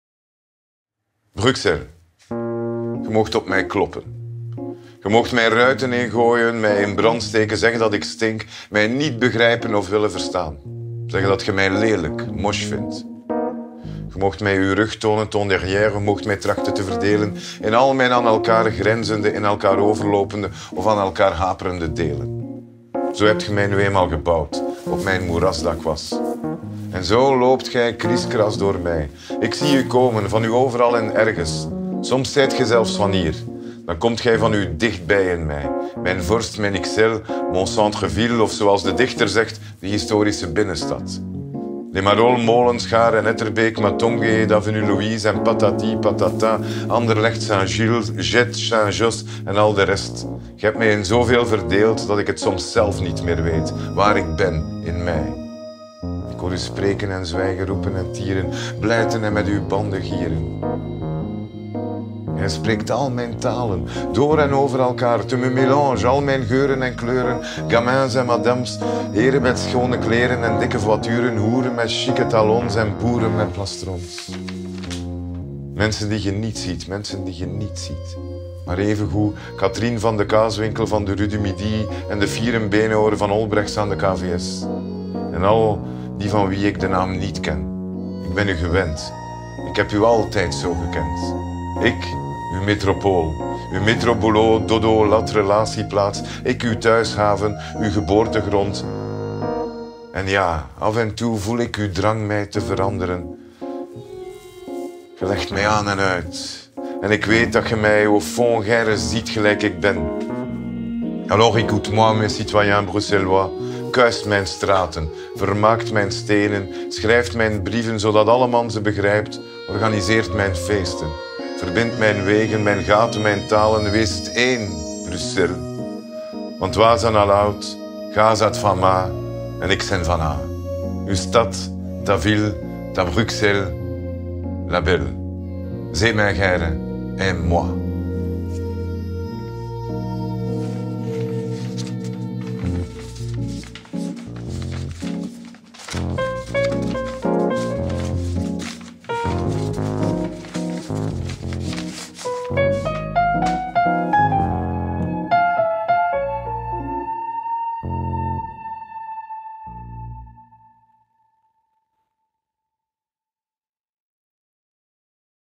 Bruxelles l Jan Ducheyne l Festival Stemmen in de Stad 2020
#6 MICROFOON "Bruxelles"
Jan Ducheyne, poëzie
Jean-Philippe Collard-Neven, muziek

Ontdek de virtuele tweetalige poëtische jam van Laurence Vielle en Jan Ducheyne, opgenomen in Het Goudblommeke in Papier / La Fleur en Papier Doré in het kader van het Festival "Stemmen in de Stad".